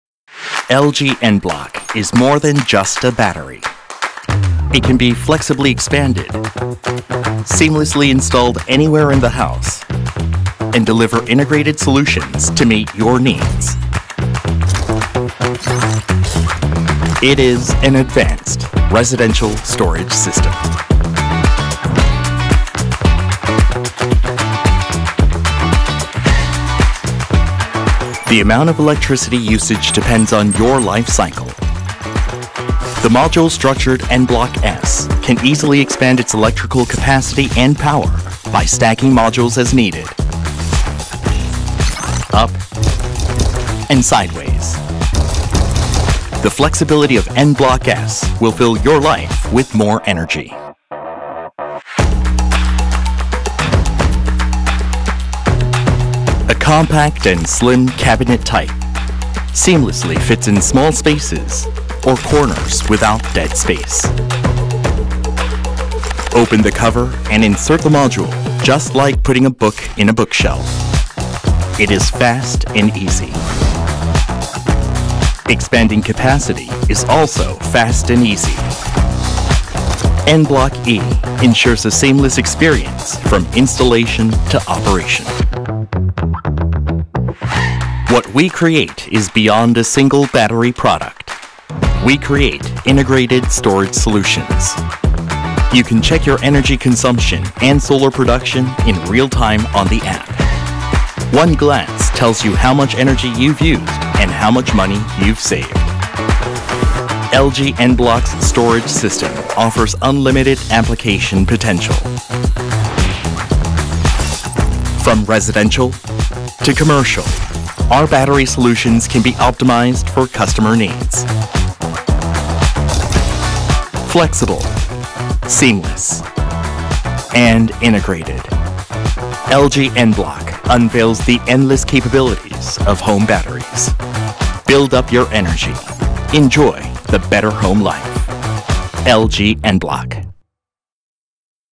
LG enblock Product Introduction Video
Introducing LG enblock, New Residential Storage System Lineups.

Flexible, seamless, and integrated LG enblock
unveils the endless capabilities of home batteries.

Build up your Energy, Enjoy the better home life.